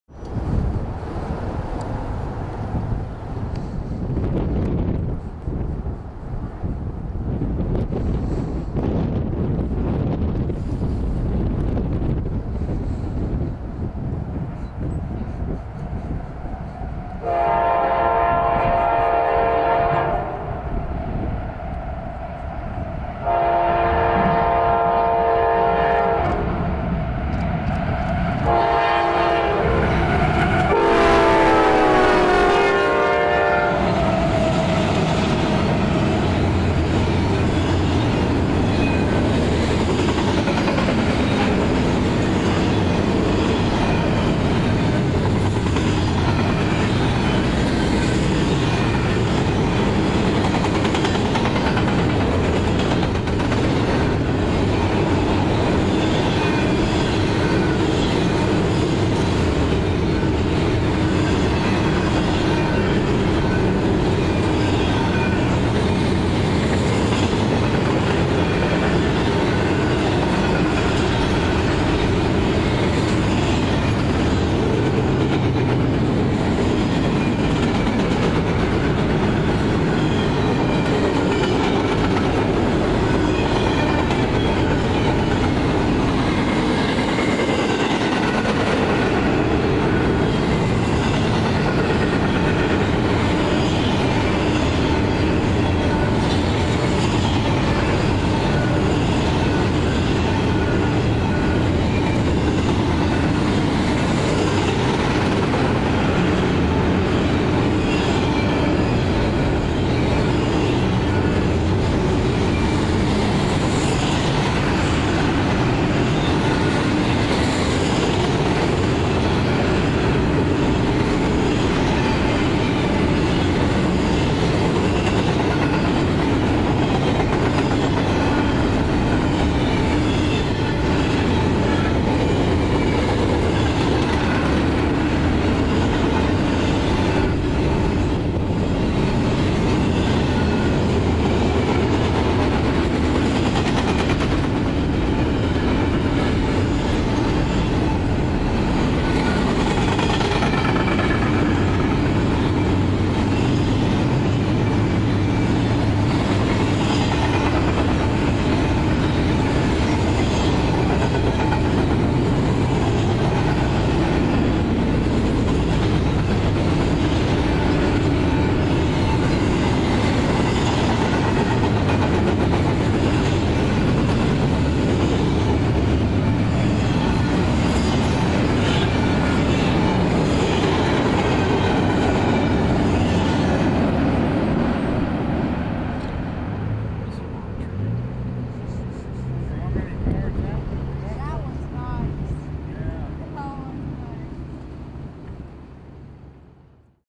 (14) Big Powered Stack Train @ Westfield Nov. 15 2015
Five GE locomotives bring this east bound CSX double stack container train past Westfield, New York. November 15 2015. Sorry for the reupload. You Tube didn't process the original upload.